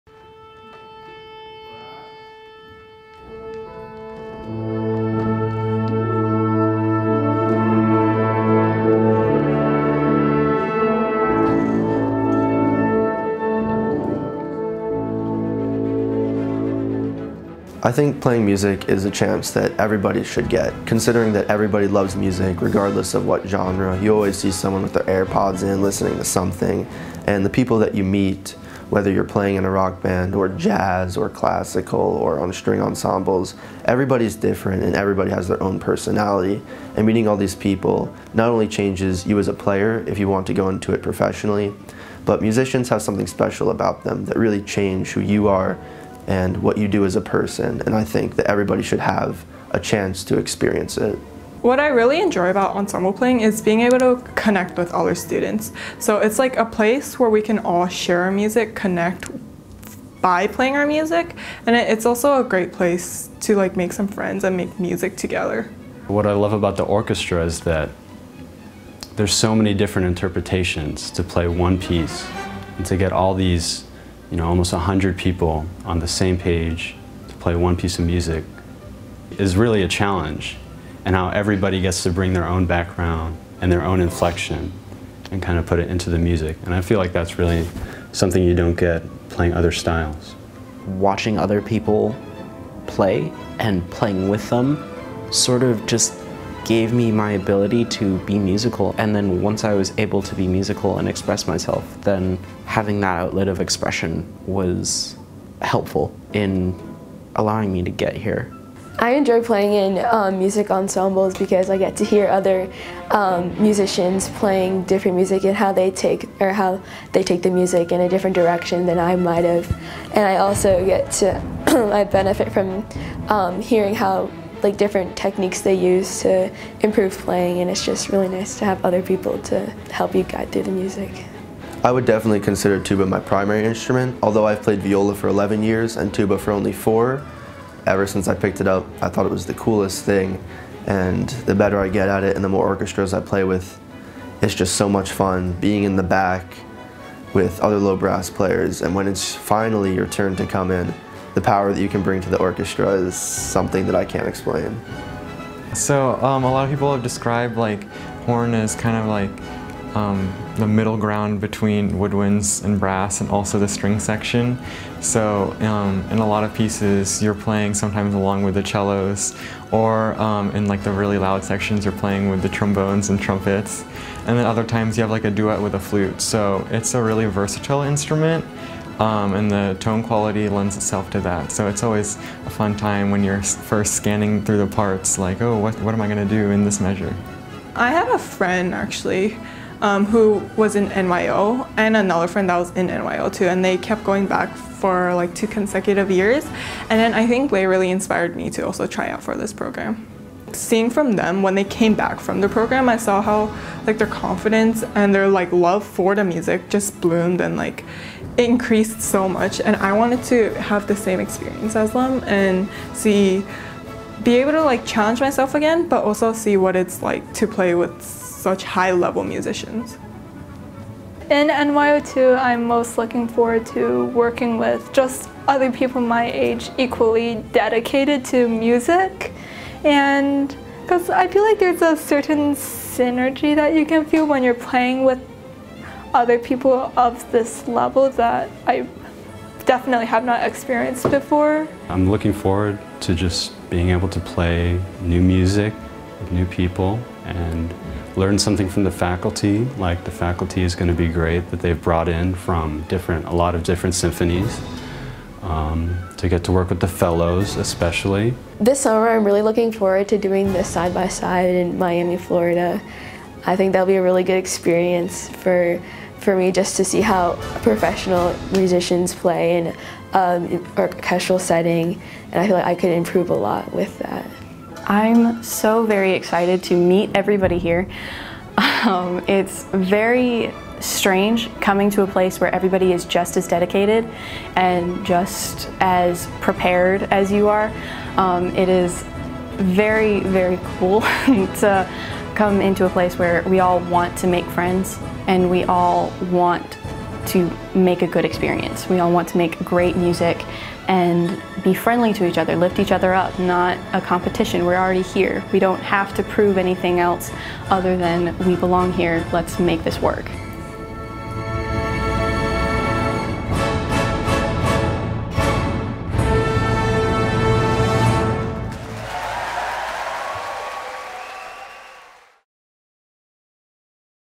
Welcome 2019 NYO2!
The 80 members of this summer’s NYO2—extraordinary teen instrumentalists ages 14–17 from across the United States—come together for a training residency at Purchase College, SUNY, before returning to Miami Beach for their second annual summer residency with the New World Symphony. The program culminates in performances at Miami Beach’s New World Center and at Carnegie Hall.